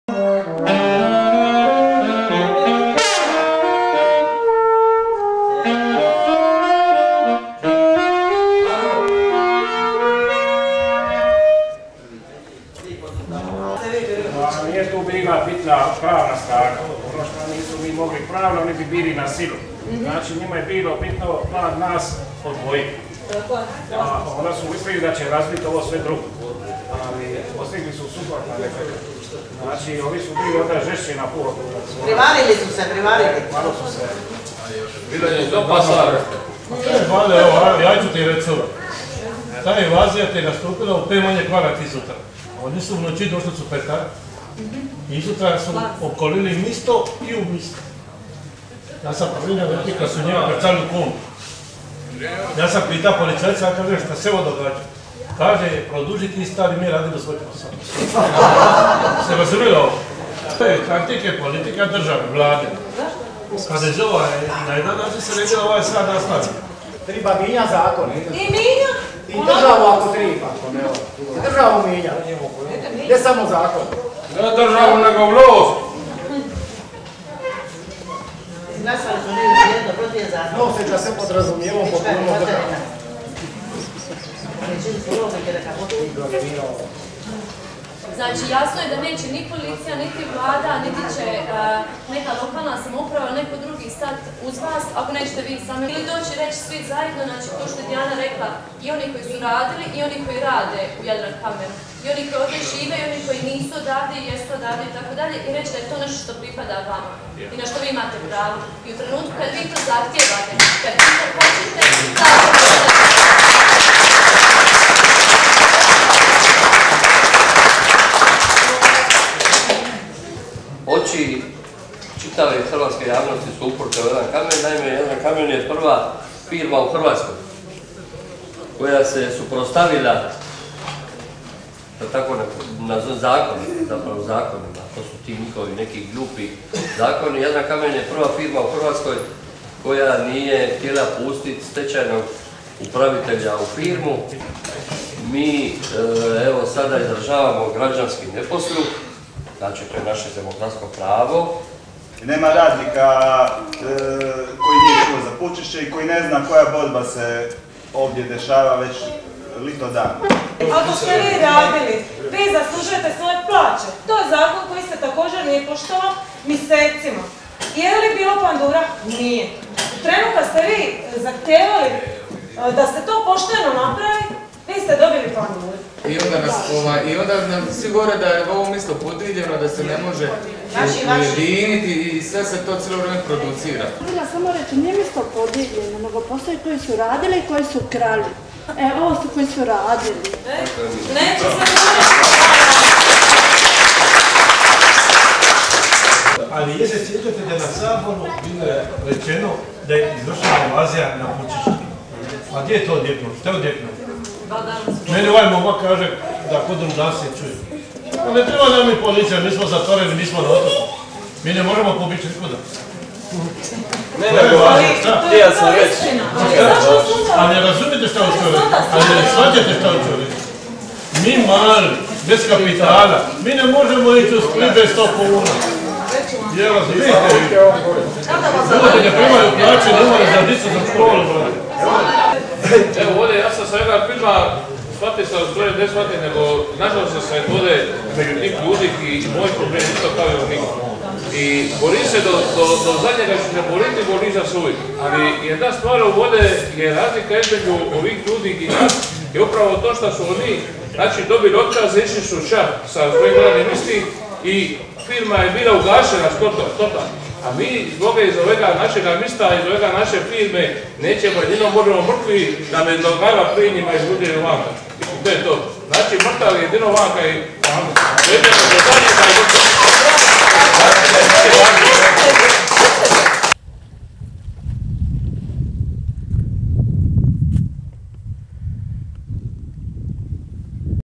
Jadrankamen tribina 16.06.2012.
Pučišća, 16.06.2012., unatoč protivljenju načelnika općine održana projekcija dokumentarnog filma "Preuzimanje" ("The Take"), art-protest i tribina podrške radnicima Jadrankamena u organizaciji Mreže solidarnosti